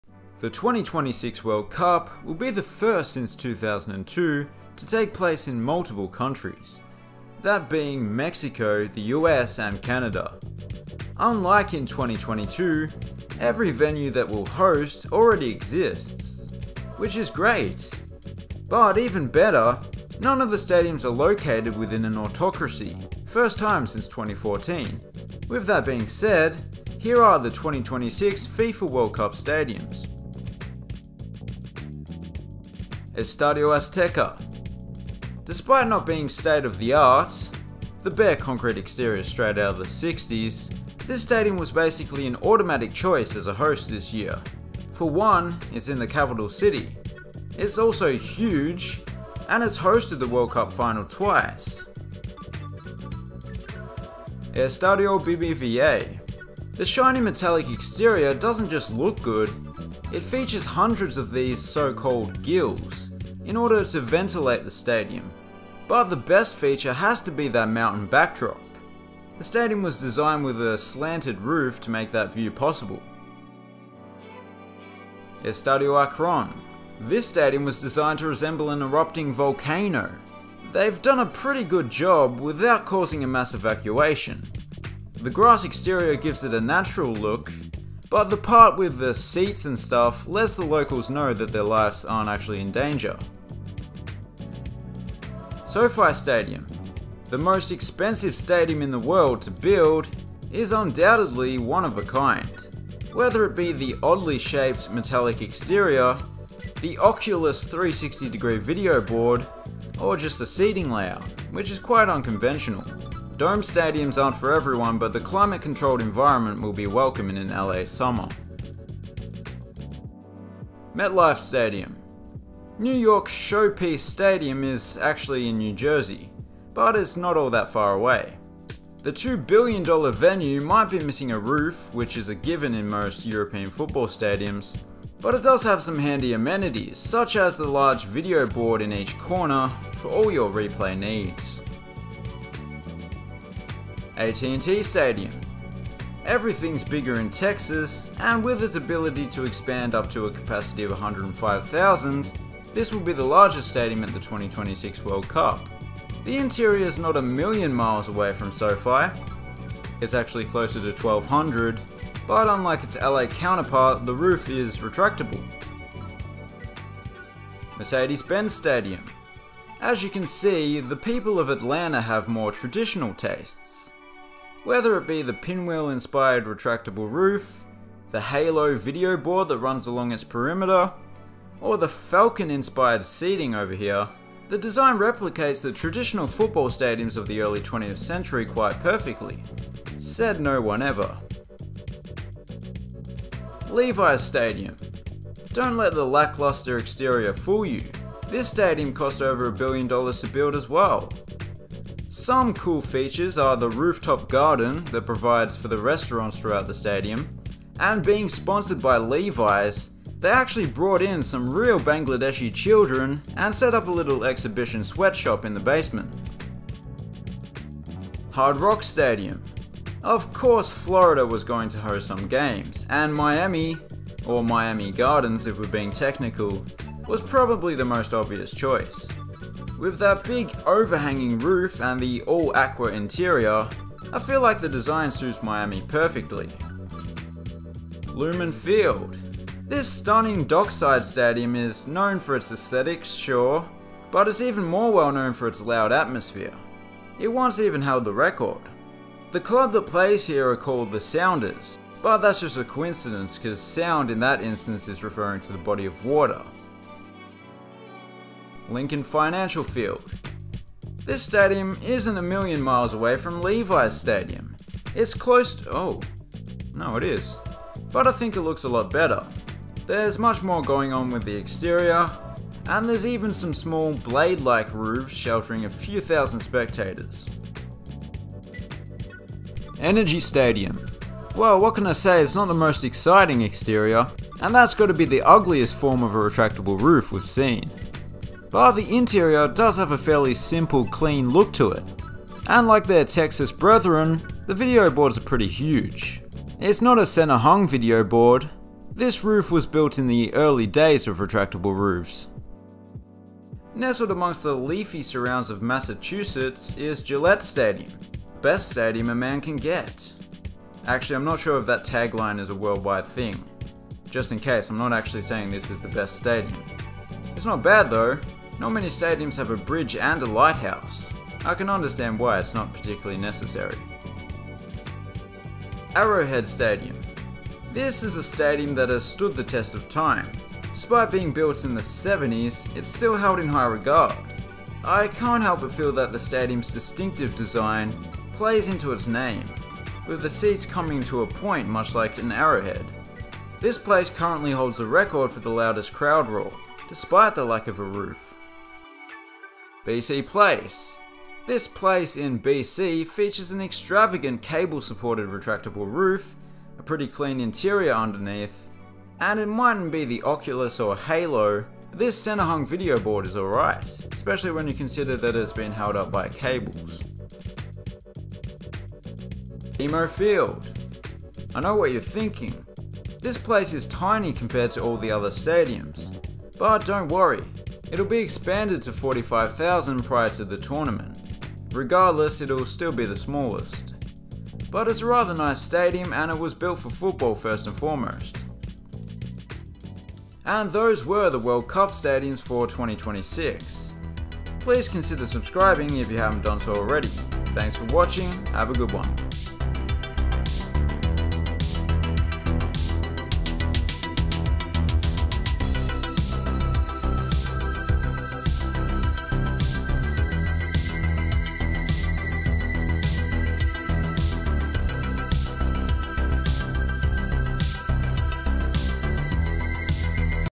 2026 FIFA World Cup Stadiums
We showcase the stadiums of the 2026 FIFA World Cup that will be held in Mexico, the US and Canada.